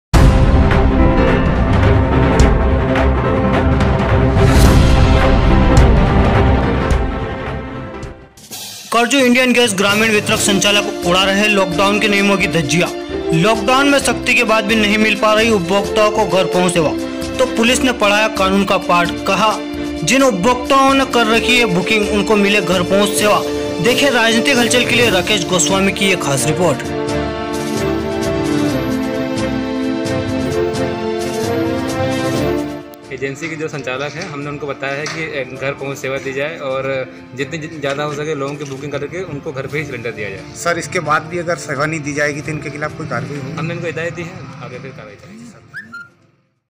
करजु इंडियन गैस ग्रामीण वितरक संचालक उड़ा रहे लॉक डाउन के नियमों की धज्जियां, लॉक डाउन में सख्ती
करजु इंडियन गैस ग्रामीण वितरक
संचालक उड़ा रहे लॉक डाउन के
नियमों की धज्जियां,
लॉक डाउन में सख्ती के बाद भी
नहीं मिल रही ग्राहकों को घर पहुंच सेवा,